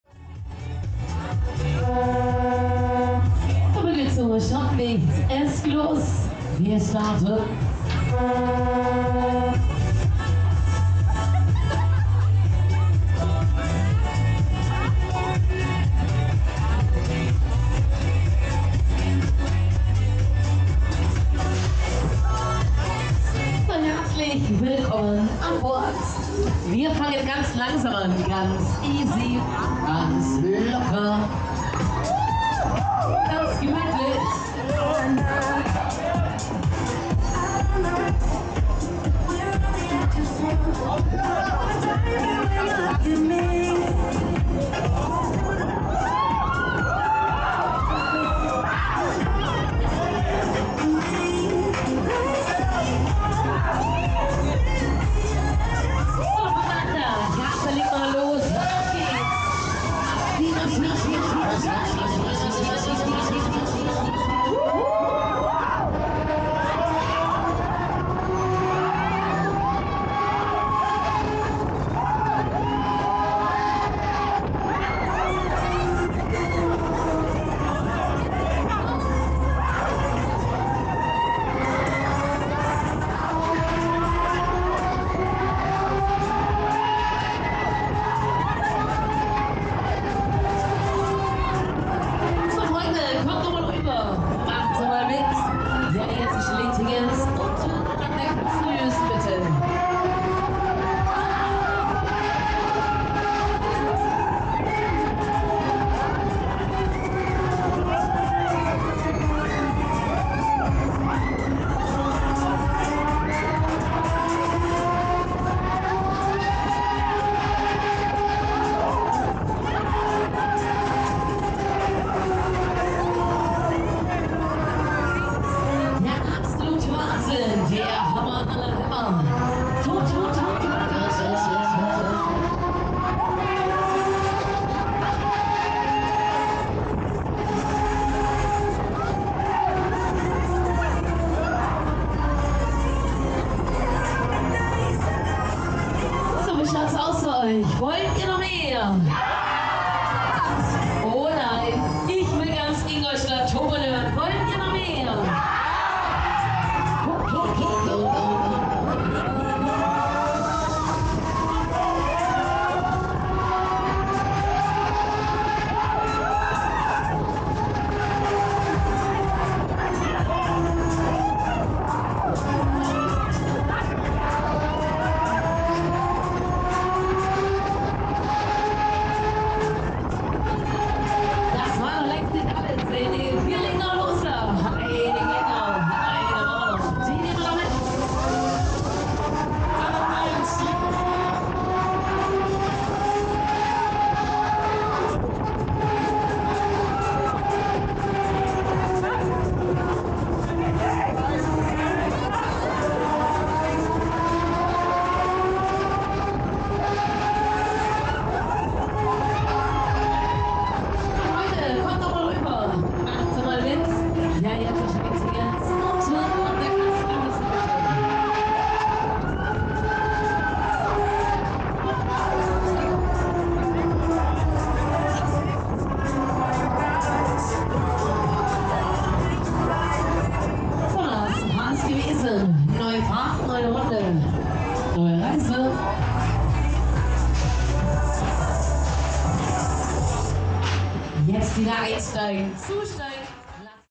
Breakdance - Zehle - Onride (POV) - 4K - Ingolstädter Volksfest 2023
I saw this Break Dance 1 by german showman Zehle already this year in Augsburg. I got some new footage here, because of one simple reason. The sound of the motor. Louder than the music! Makes it not only great for uploading (no content ID claims) but also because it makes the ride feel more powerful. Good looking design, too! The ride op was fantastic, smiling pretty much all the time and she knew how to control this ride. When you leave the Break Dance at any speed constantly, the gondolas start to spin the same way every rotation and it gets repetetive. Slowing the main plate down and getting it back up to full speed make the gondolas spin pretty unpredictably and fast.
TLDR: Good one :)